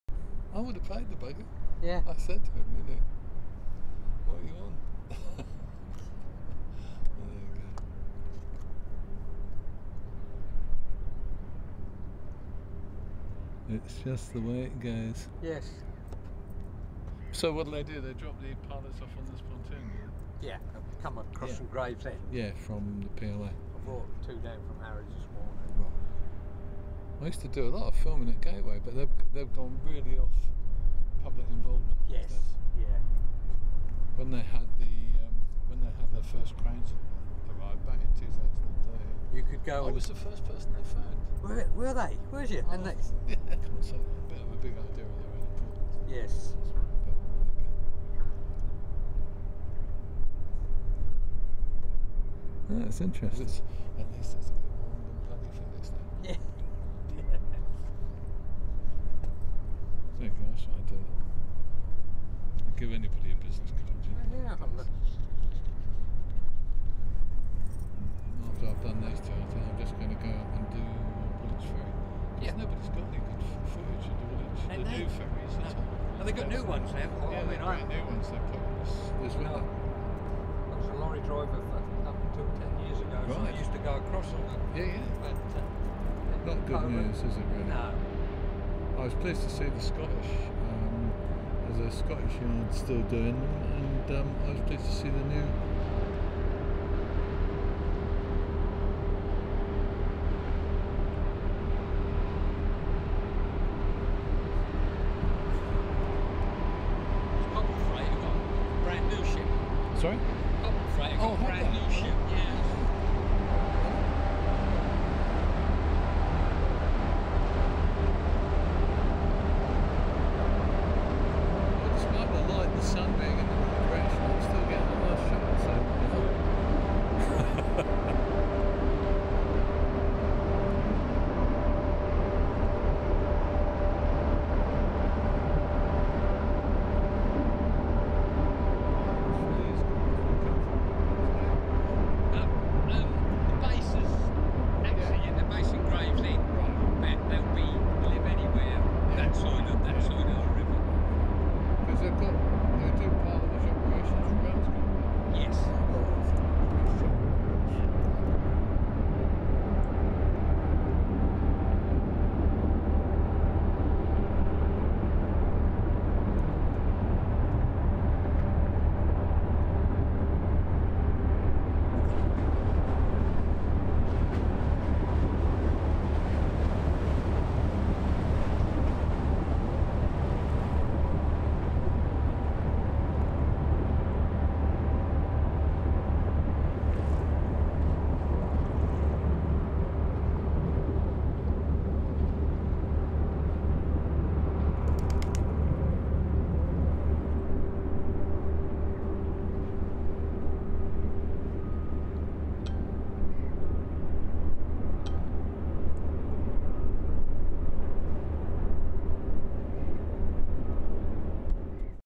Feeder Pengalia arrives at Tilbury, 10 May 2019
The 690 TEU feeder Pengalia passes Gravesend on her way to Tilbury Dock on the Thames, approaching low water on Friday, 10 May 2019.